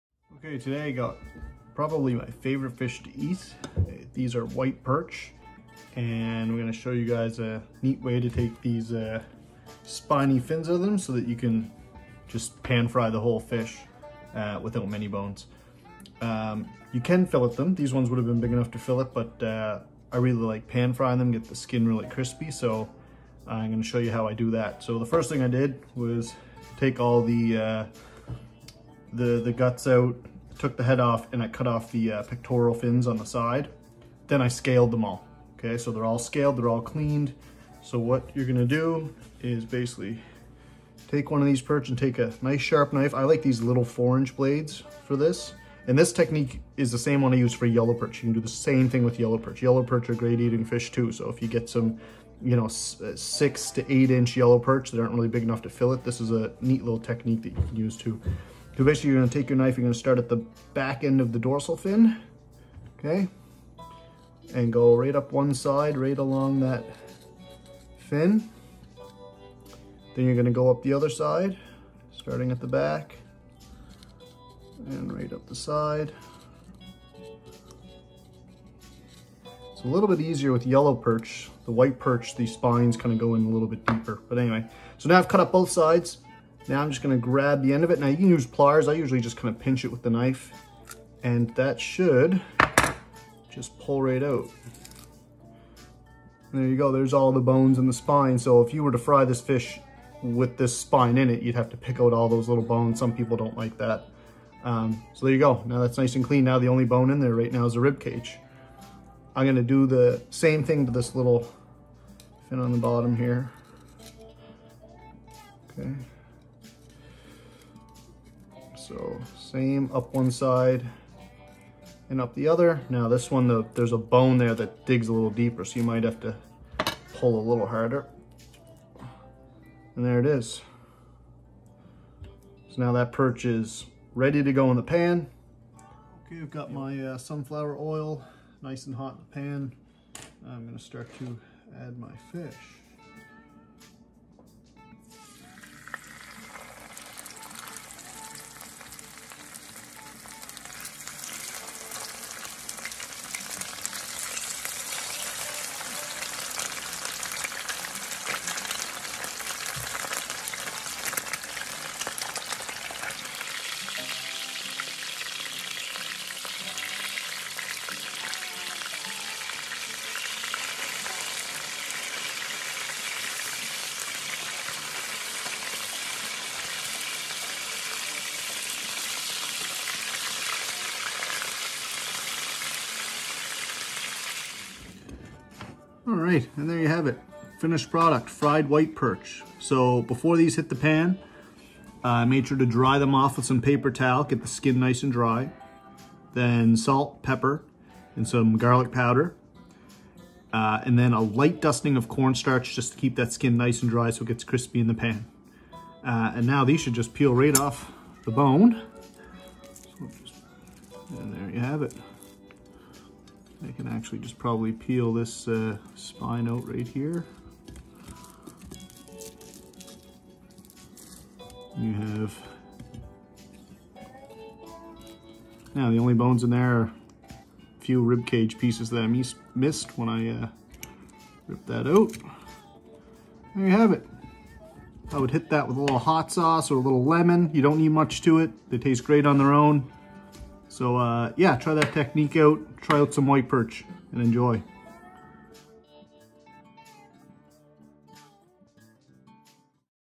FISH EAST | Matt Szeto | Fried White Perch
The first of many catch and cook videos to come, Matt Szeto demonstrates an alternative method to prepare white perch for the frying pan.